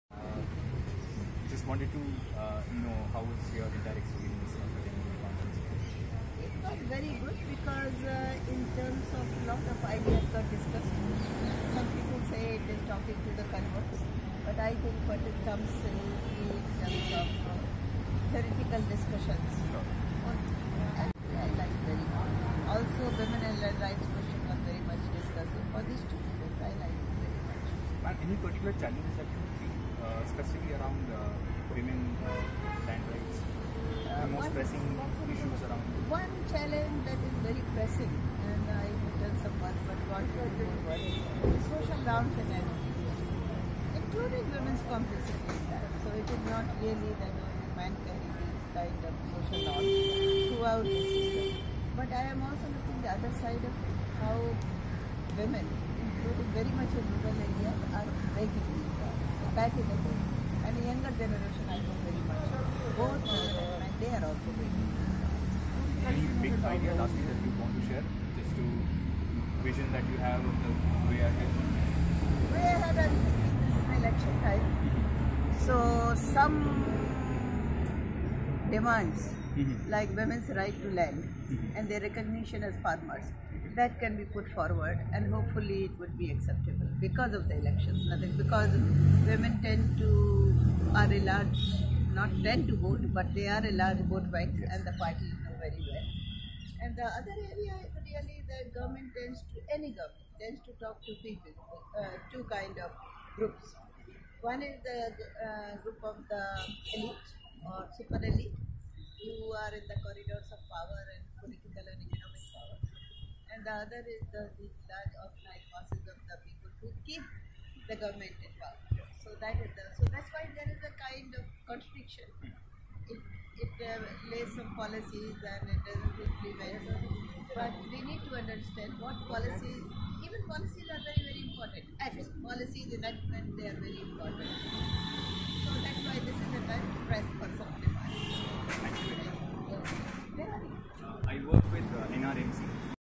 Interview with Dr. Govind Kelkar at the India Land and Development Conference on Women's Land Rights
Govind Kelkar is Senior Advisor to Landesa in New Delhi, India. She is also Regional Council Member of Asia-Pacific Forum on Women, Law and Development, Chiang Mai, Thailand. She has previously worked for UN Women, and taught at Delhi University, the Indian Institute of Technology, Mumbai, and the Asian Institute of Technology (AIT), Bangkok, Thailand. At AIT, Dr Kelkar founded the graduate program in Gender Development Studies and also the Gender, Technology and Development Journal, published by SAGE, India. Govind has worked extensively on gender and energy transition in rural Asia. She has contributed numerous articles to scholarly journals with a focus on gender relations in Asia and has been in close touch with women’s movements in the region.